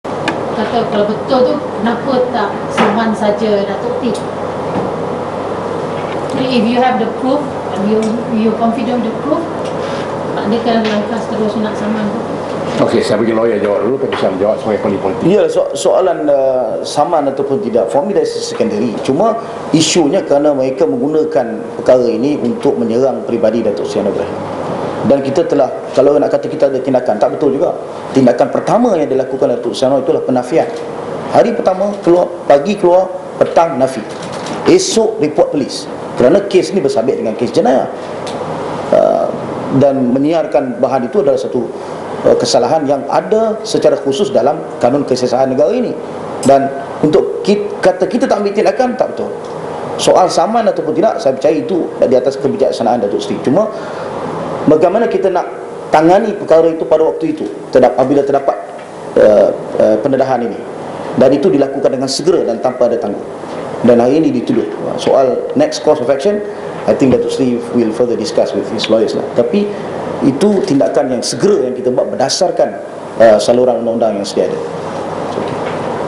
Kenapa tak saman sahaja Datuk T?
Datuk Johari Abdul ditanya oleh wartawan
kenapa tidak saman sahaja Datuk T jika mempunyai bukti dan ini jawapan
beliau...

24 Jun 2011

2.30 petang

Ibupejabat PKR